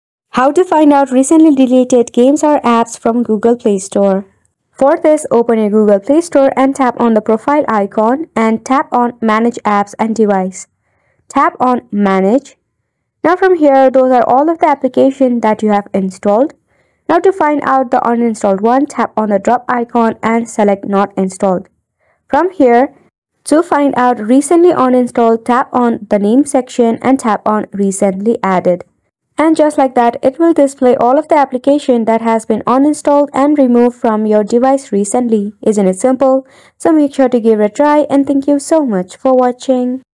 How To Find Out Recently Deleted Apps/Games from Google Play Store History
In this tutorial, we'll guide you through the process of finding out recently deleted apps or games from your Google Play Store history. Google Play Store maintains a record of your installed apps and games, and we'll show you how to access and view this history.

Tips for Finding Recently Deleted Apps/Games in Google Play Store History:
🔗 Accessing your app and game history can be useful for keeping track of your downloads. We'll walk you through the steps to access this information.

📱 Timestamps:
0:00 - Introduction
0:05 - How to Find Out Recently Deleted Apps/Games from Google Play Store History
0:10 - Step-by-Step Guide to Accessing History